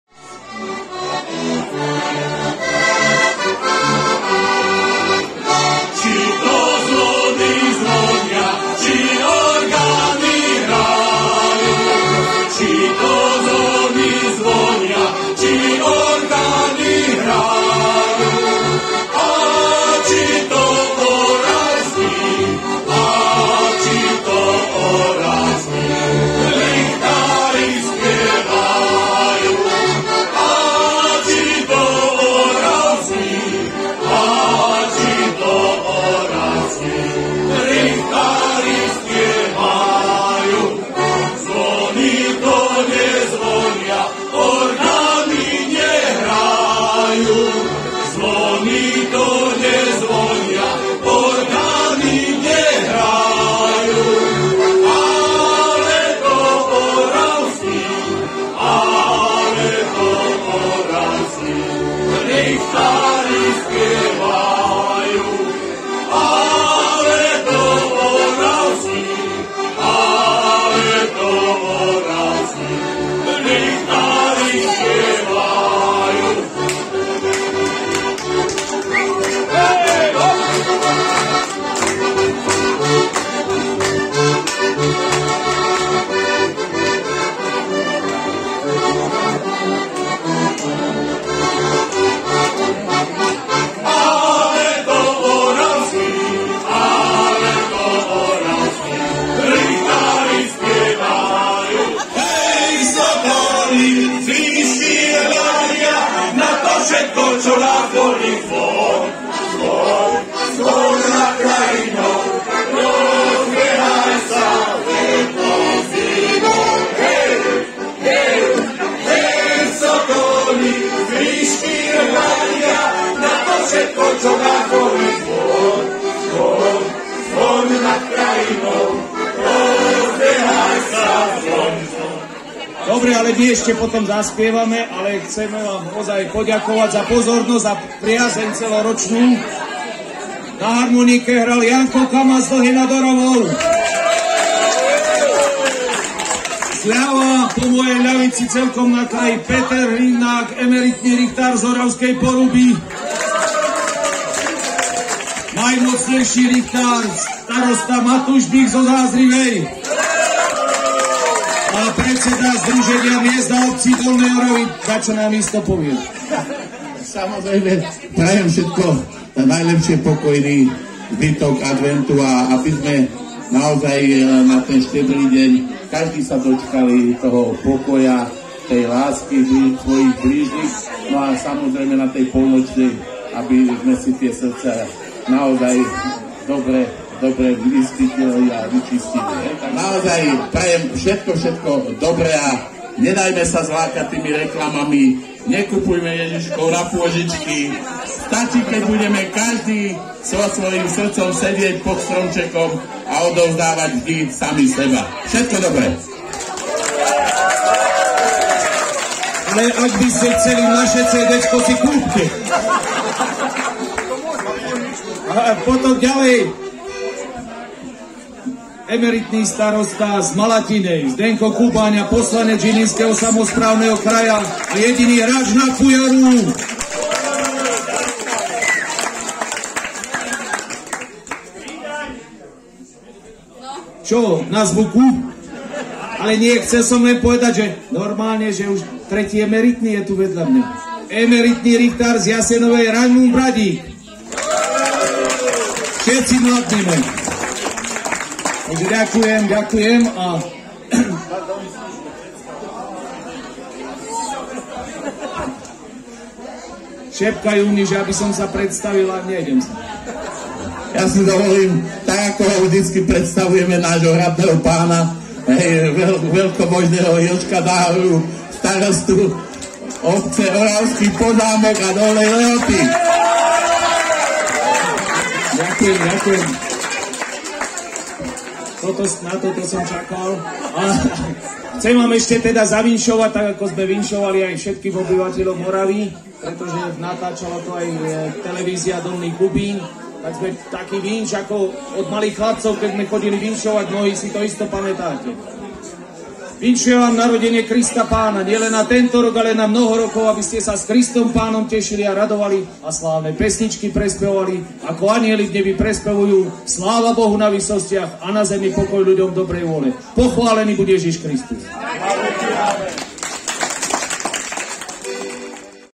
Vianočné trhy pod Hradom v Oravskom Podzámku 17 12 2022
Oravskí richtári na podujatí Vianočné trhy pod Hradom v Oravskom Podzámku dňa 17. decembra 2022